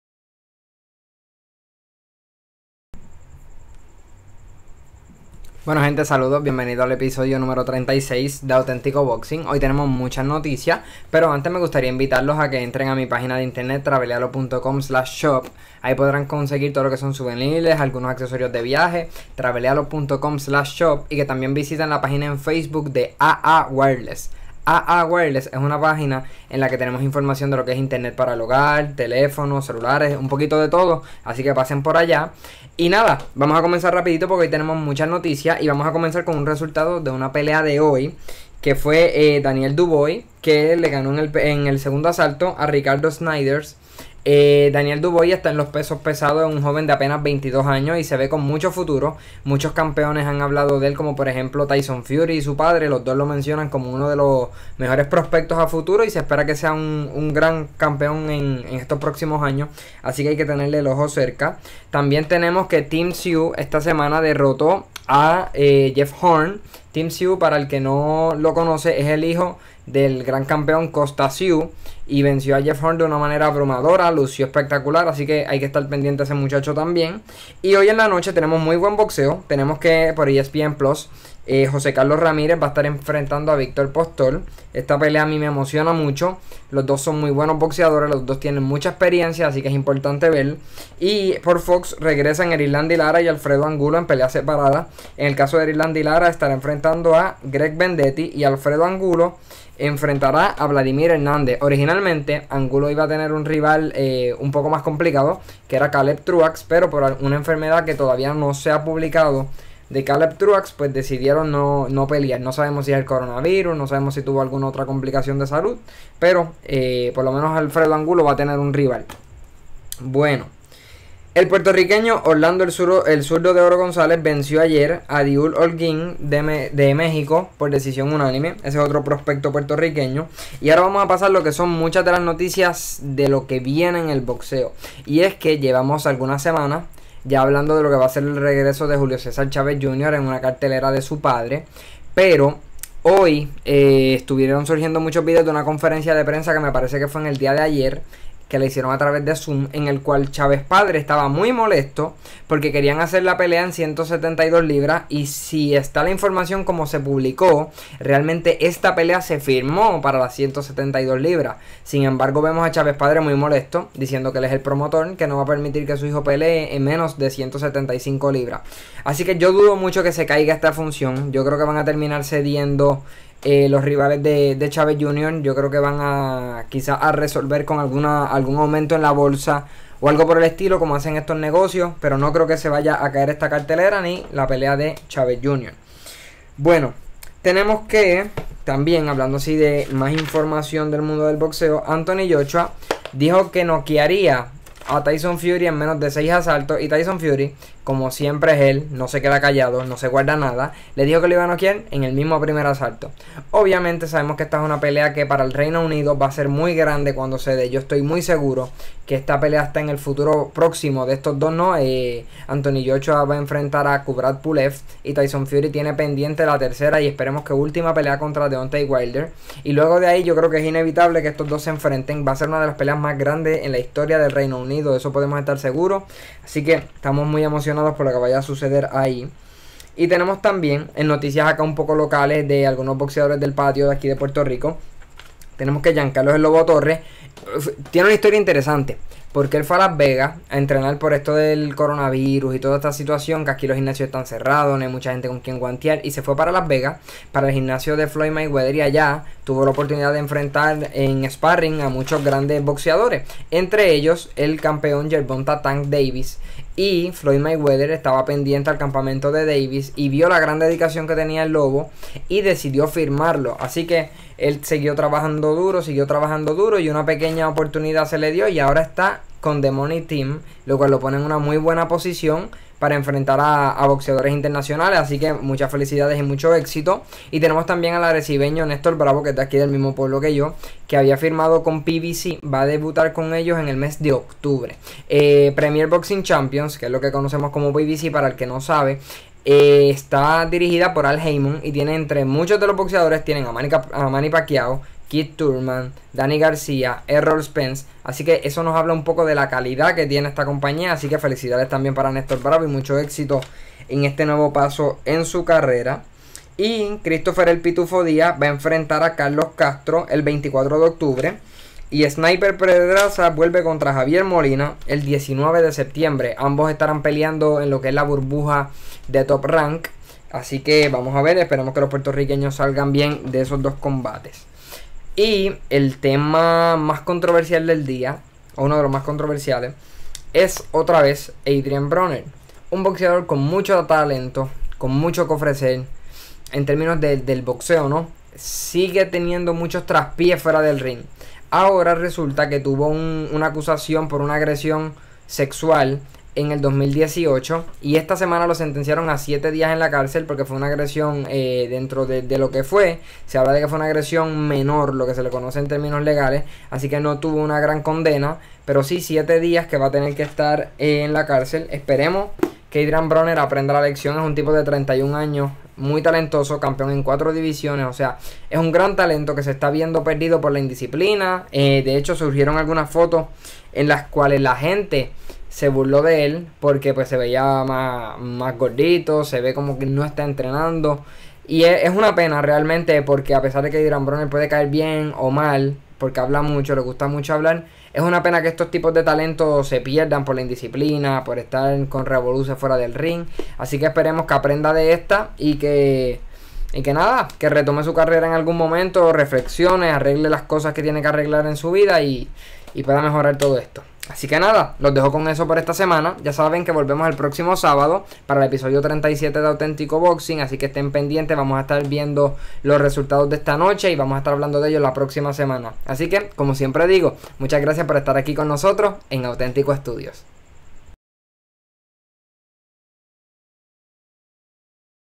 CHÁVEZ PADRE DEFIENDE A SU HIJO, Adrien Broner a la cárcel y mucho más | Auténtico Boxing Ep. 36
Temas para hoy:
- CHÁVEZ PADRE DEFIENDE A SU HIJO
- Adrien Broner a la cárcel

SÍGUENOS
Alex Candelario